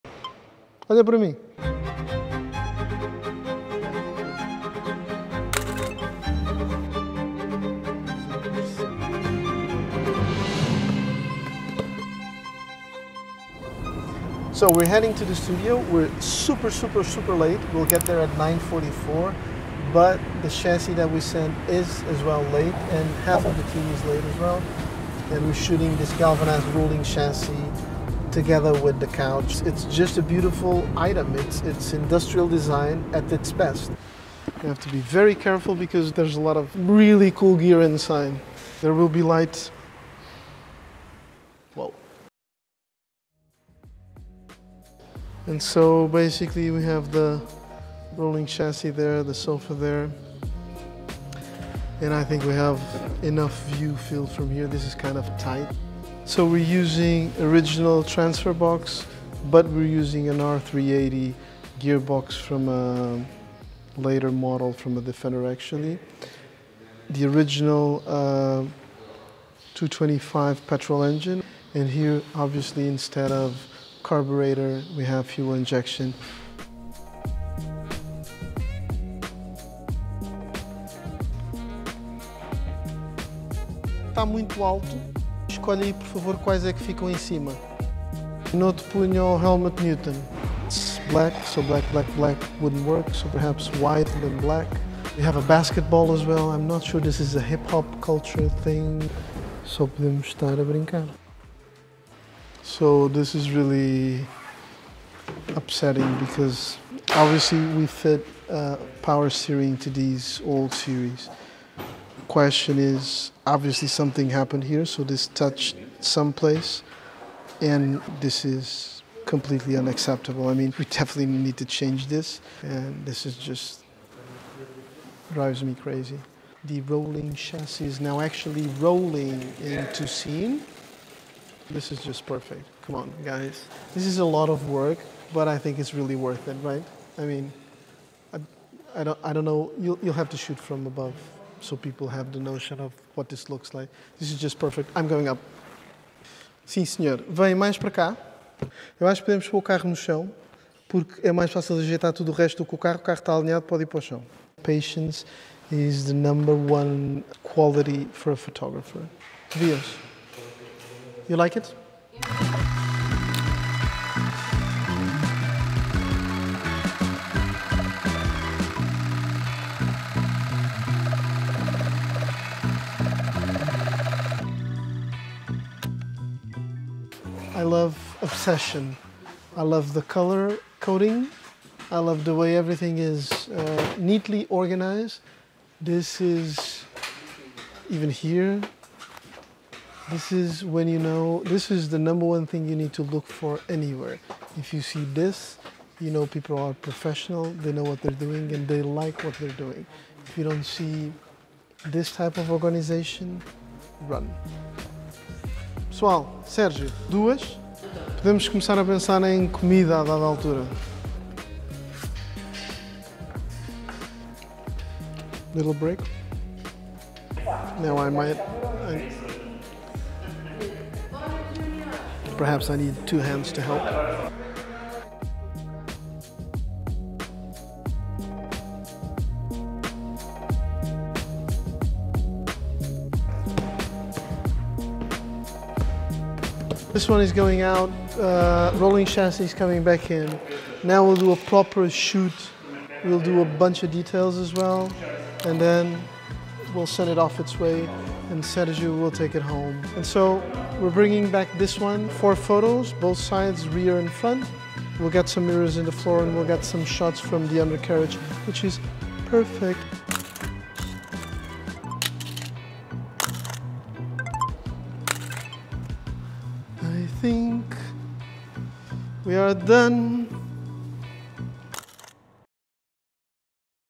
Studio Chronicles - Land Rover Series 3 + Camaleonda B&B Italia
The timeless design of Mario Bellini's Camaleonda sofa meets the rugged charm of a restored Land Rover Series 3 chassis.
An intimate journey with each piece telling its own story of design, innovation and adventure.
It's a straightforward look at how we photograph these timeless pieces, highlighting their design and the attention to detail that goes into each  of our builds.